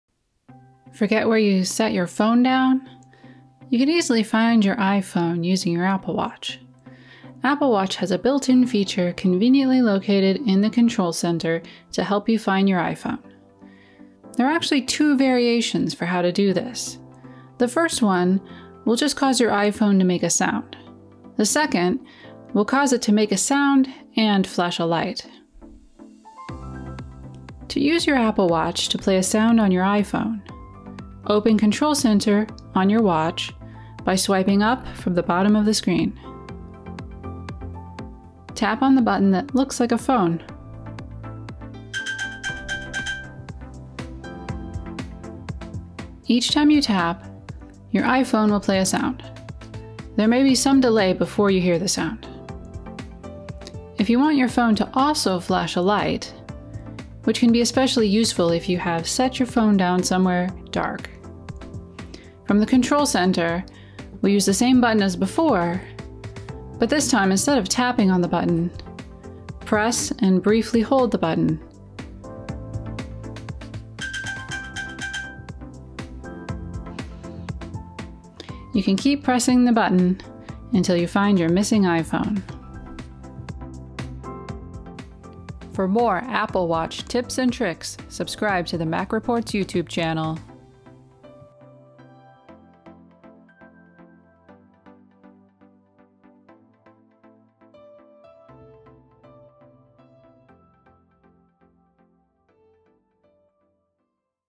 How to Ping Your iPhone with Your Apple Watch
You can find your iPhone using your Apple Watch. The Apple Watch has a built-in feature that lets you play a sound and flash a light on your iPhone to help you locate it.